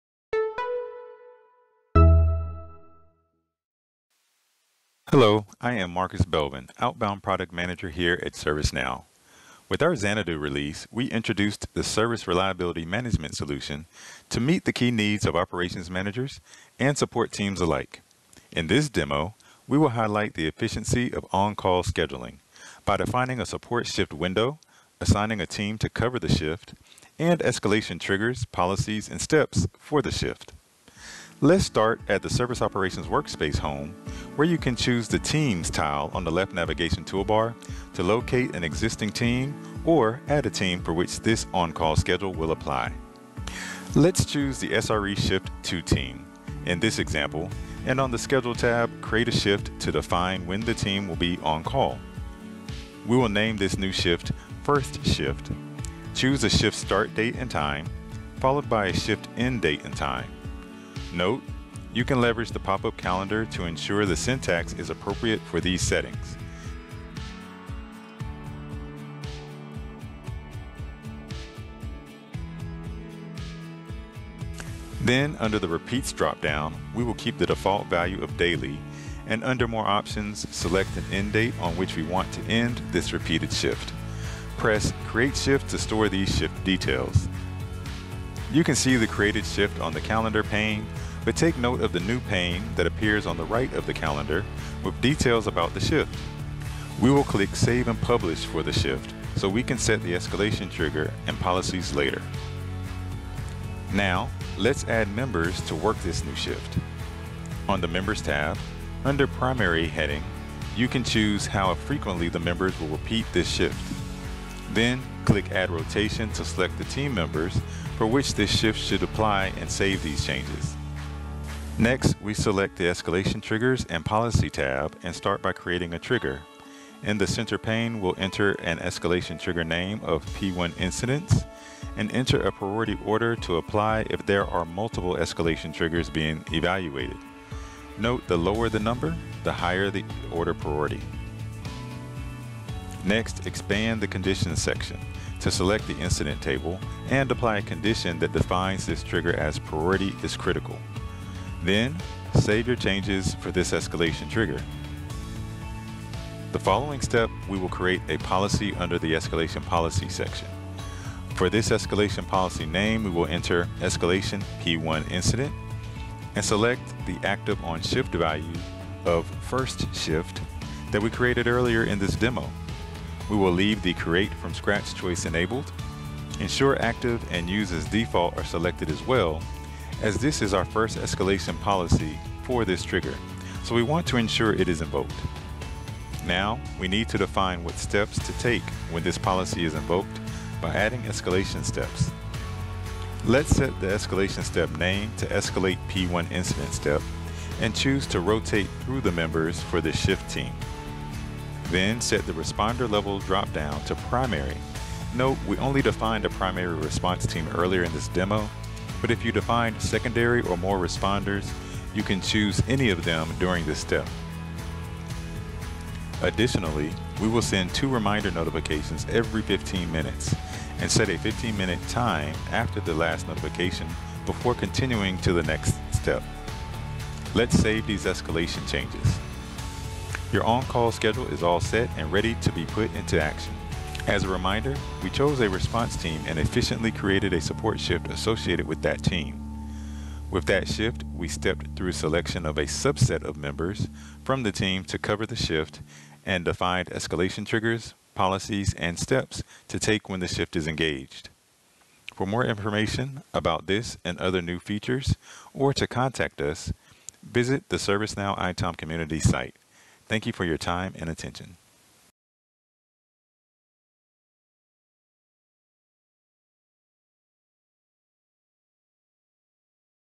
Demo: Service Reliability Management - On-call scheduling
In this Service Reliability Management (SRM) demo, we show the ease of on-call scheduling which is designed to enhance service resilience and reliability by creating schedules that provide 24x7 on-call coverage for the production environment. It ensures that high-severity incidents requiring immediate human attention are promptly alerted to the team, thereby reducing Mean Time to Acknowledge (MTTA) and Mean Time to Resolve (MTTR). The tool also supports automated scheduling and integrated communication tools, which streamline the incident response process and improve operational efficiency.

Author: Marcus Belvin, Staff Outbound Product Manager, ServiceNow